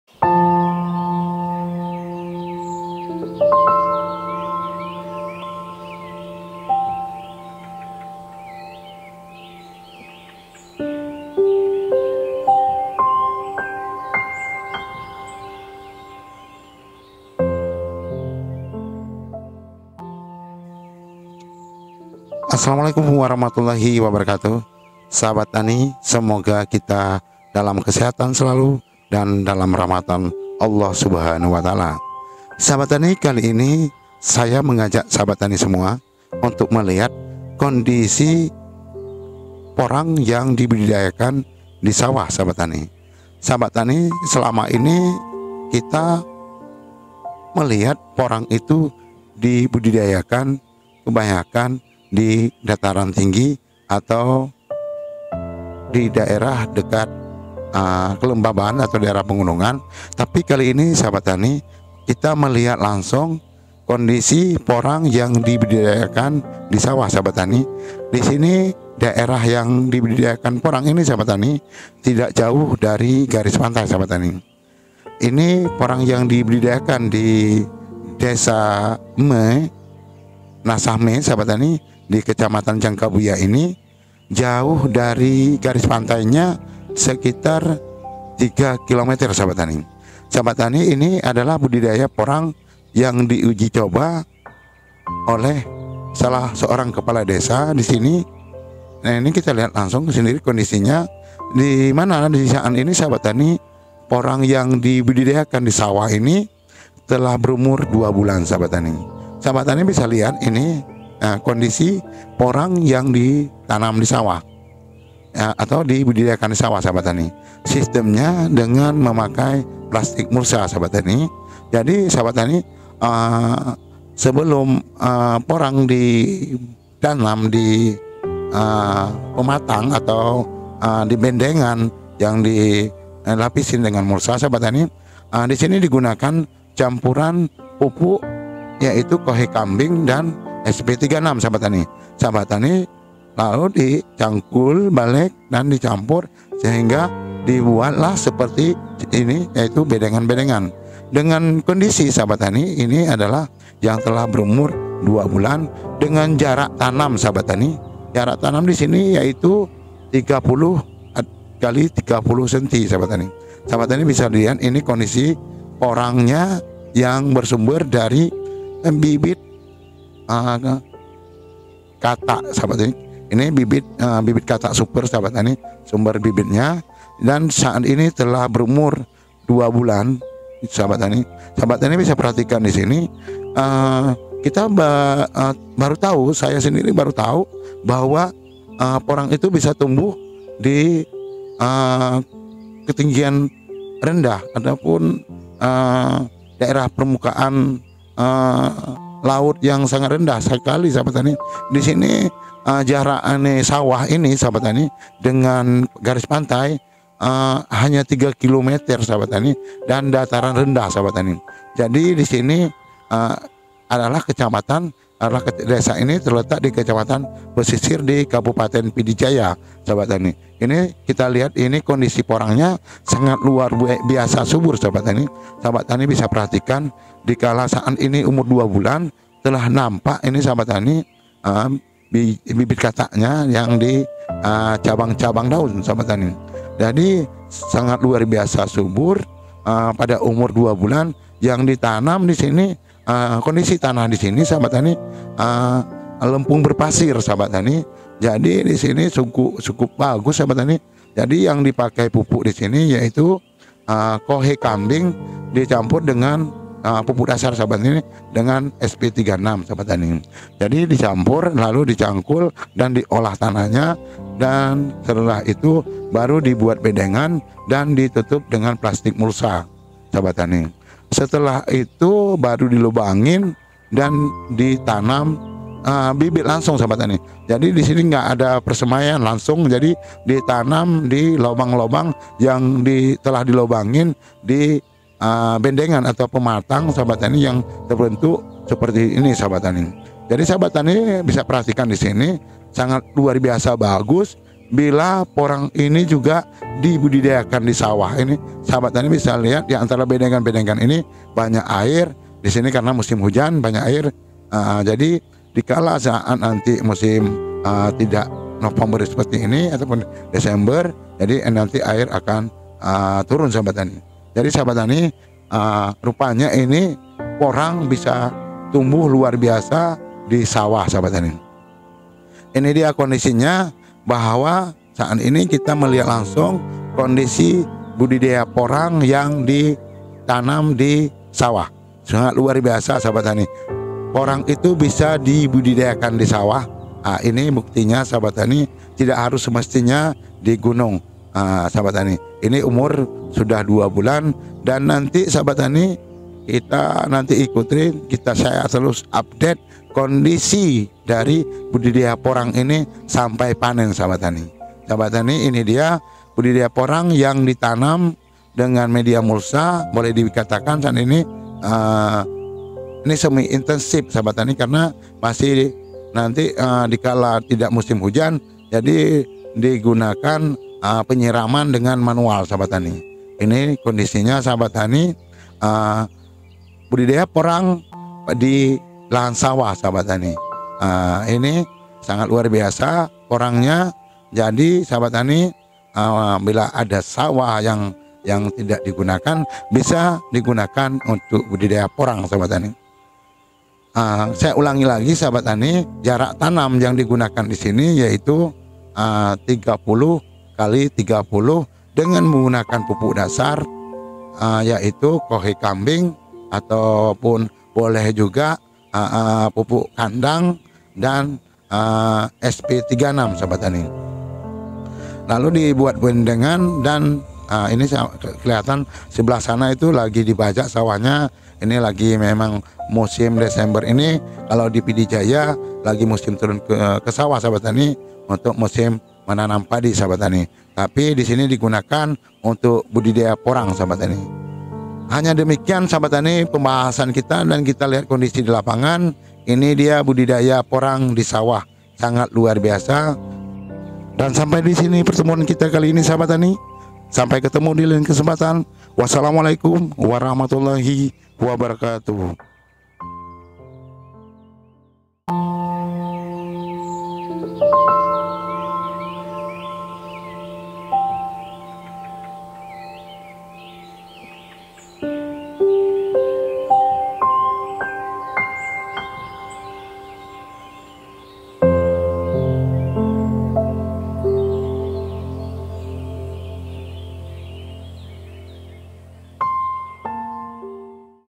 Budidaya Porang Disawah Sistem Semi Intensif
Sahabat Tani.
Kali ini Kami Persembahkan Sebuah Vidoe Tentang Budidaya Porang Disawah  dengan Sistem Semi Intensif, semoga menjadi bahan perbandingan untuk sahabat tani dalam usaha Budidaya Porang yang akan sahabat tani lakukan.

Dengan kerendahan hati kami menerima Kritikan dan Saran agar kelak dapat menjadi yang terbaik untuk kita semua.

Salam Tani.